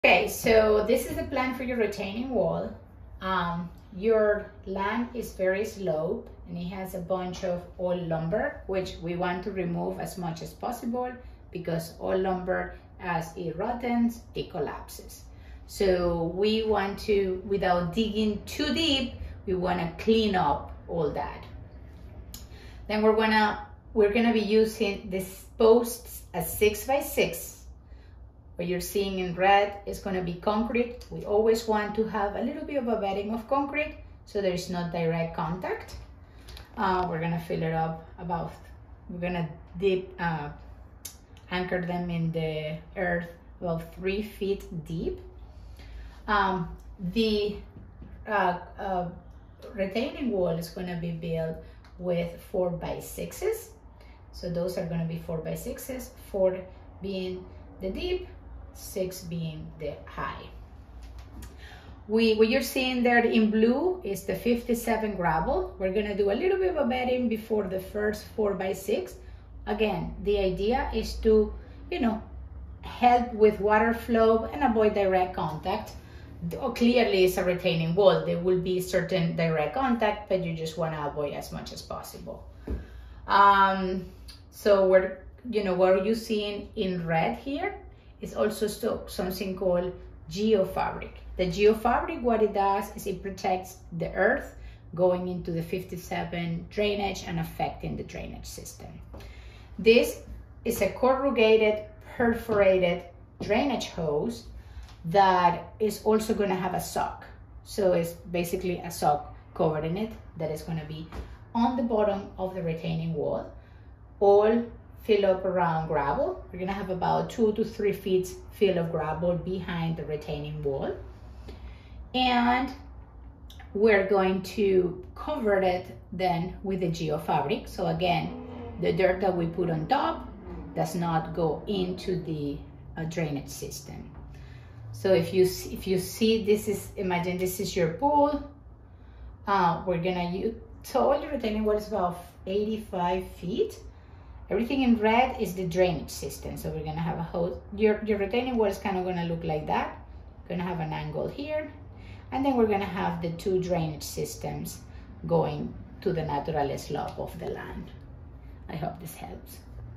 How To Build A Retaining Wall
Our construction project manager Lena describes a swimming pool retaining wall she plans to build for a client in northeast Atlanta area. This retaining wall will not only hold back the weight of the land, it will shuttle damaging water away from the wall by way of a french drain at the base of the wall. Such walls can greatly extend the life of your swimming pool deck by reducing settlement of the land surrounding the pool.